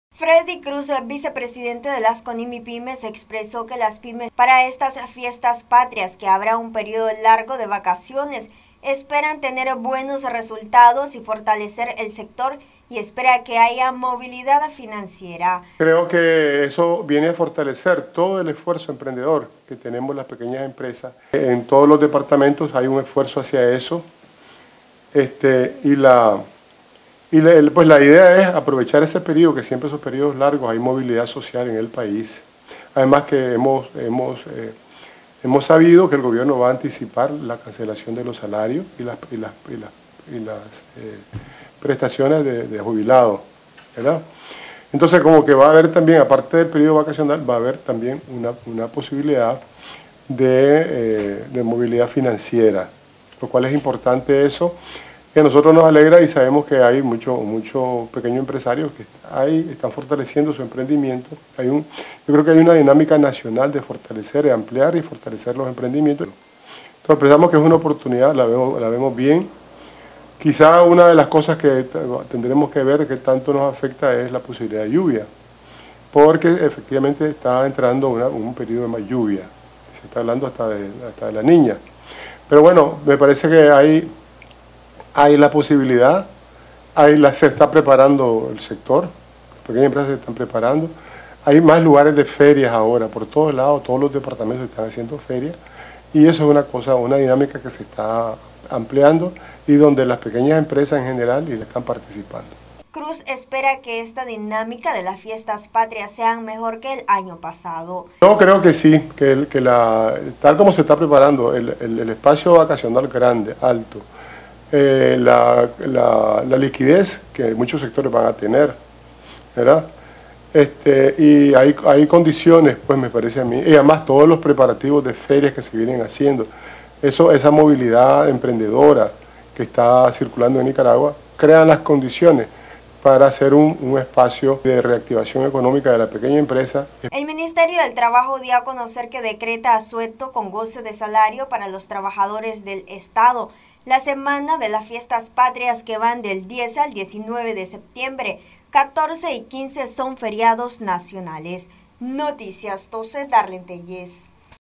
Pequeñas empresas con expectativas en fiestas patrias
Noticias12Nicaragua Vicepresidente de las Conimipymes dio a conocer que algunas pymes esperan tener buenos resultados en estas fiestas patrias en comparación al año pasado.